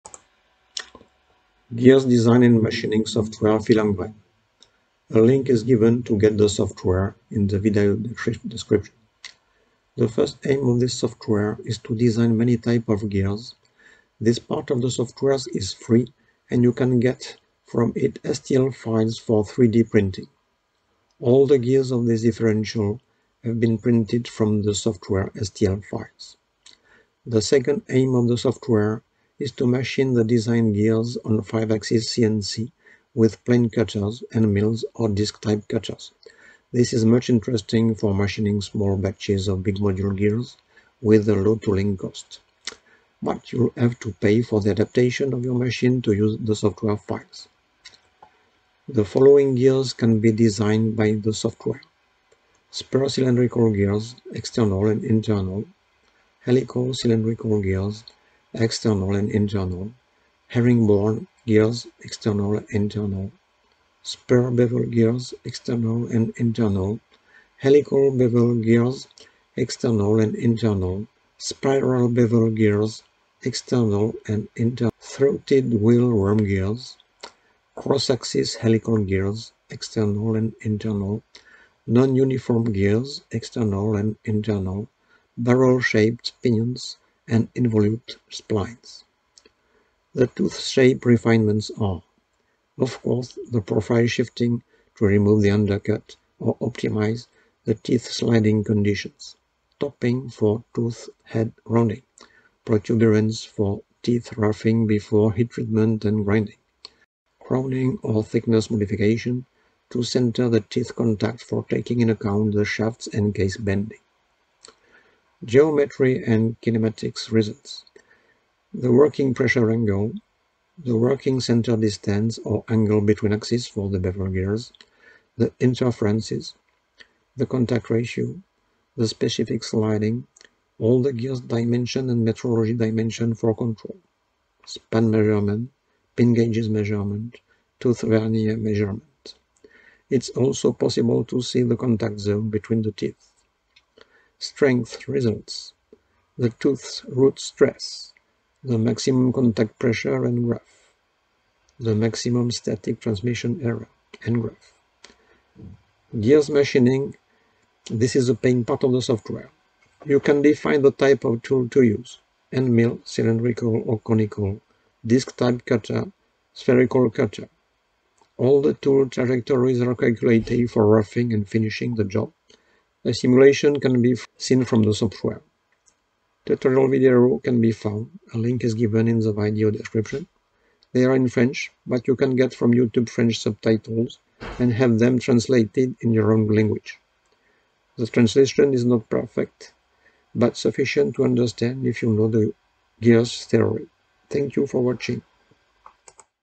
Free Software for Gear Design & Machining by Filengrene
This software is free for conception and 3D printing. But paying for machining on 5 axis CNC machines with standard milling cutters.